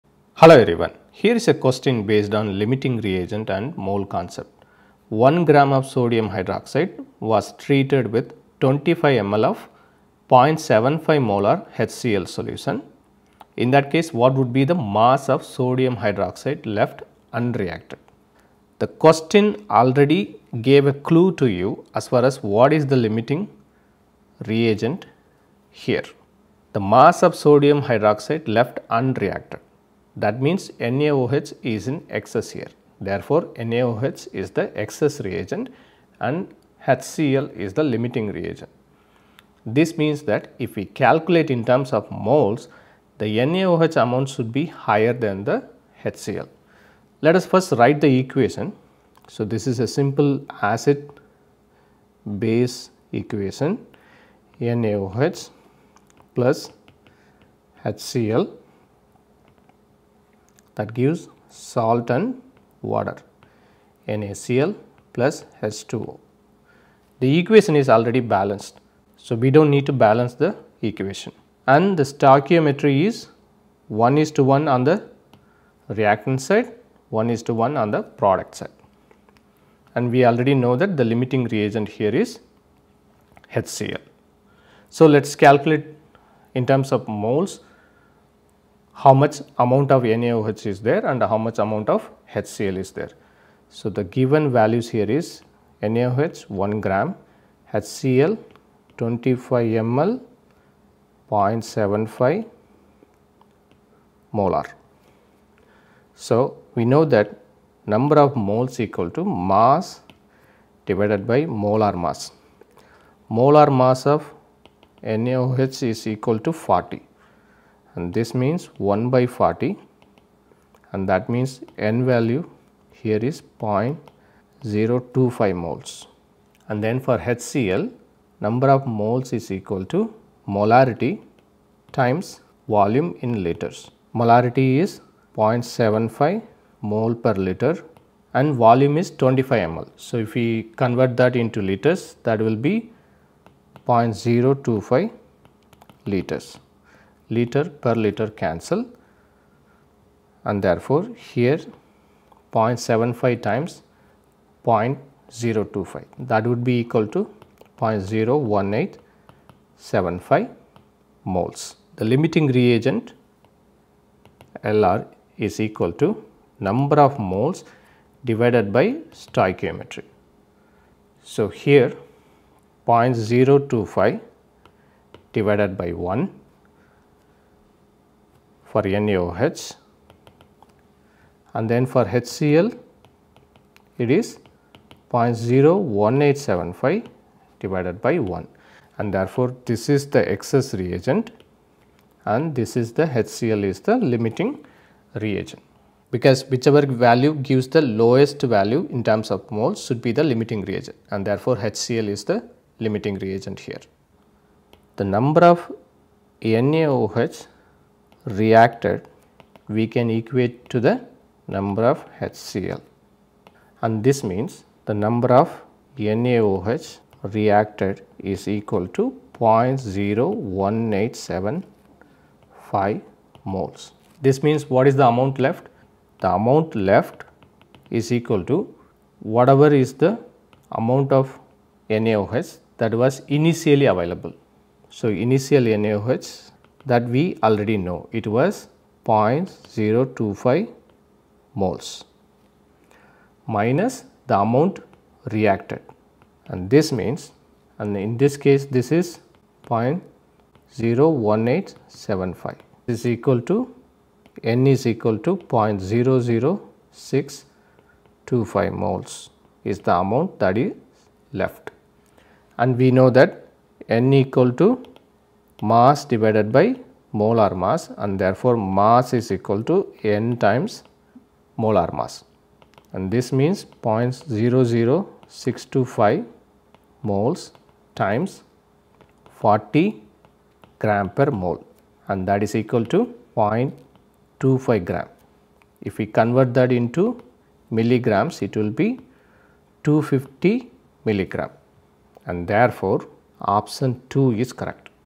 1 gram of sodium hydroxide was treated with 25 mL of 0.75 M HCl solution, the mass of NaOH left?
1 gram of sodium hydroxide was treated with 25 mL of 0.75 M HCl solution, the mass of sodium hydroxide left unreacted is equal to

(1) 750 mg
(2) 250 mg
(3) Zero mg
(4) 200 mg